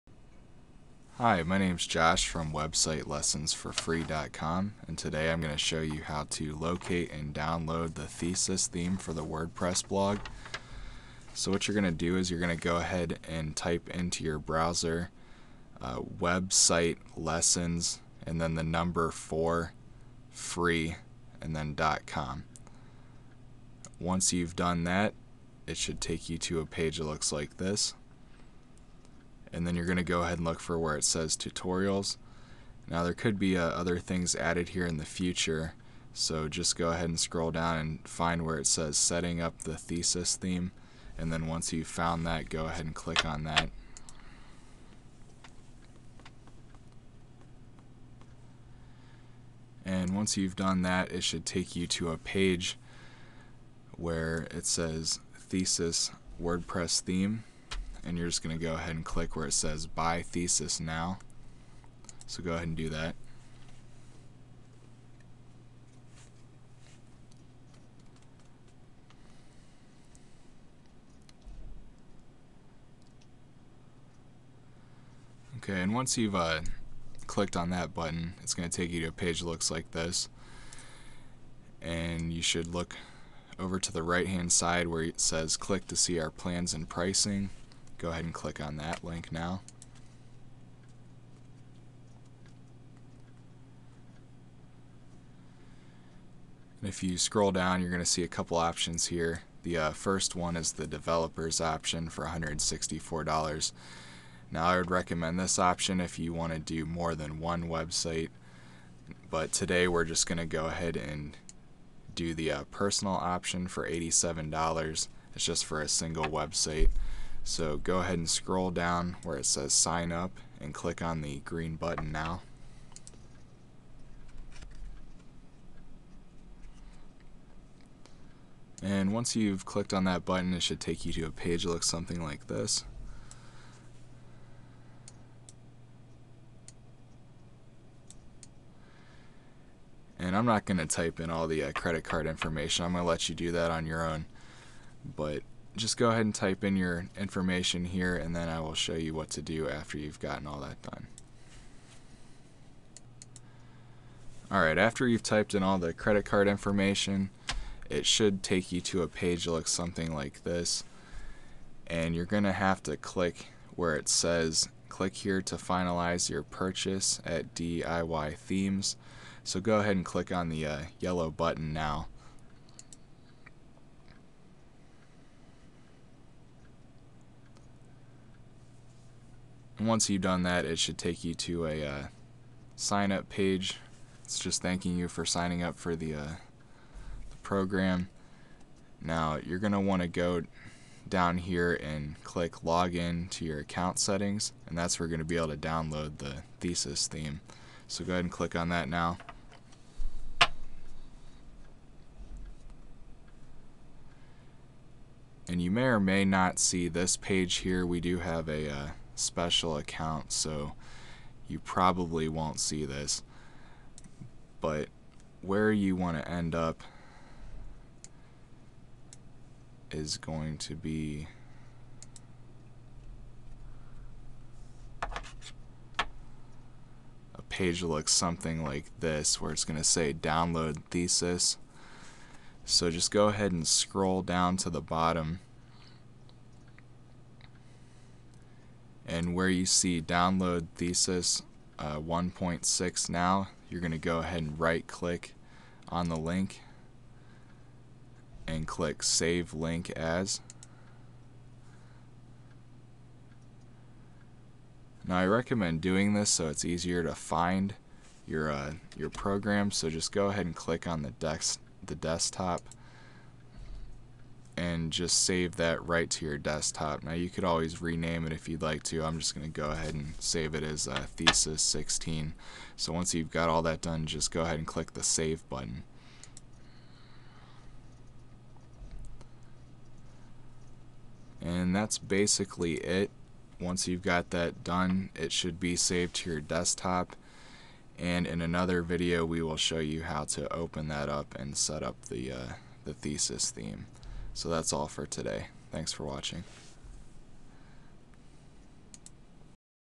Downloading the Thesis Theme for Wordpress
How to locate and download the Thesis Theme for your Wordpress Blog. This theme works great for blogs or just for creating a nice platform for your website.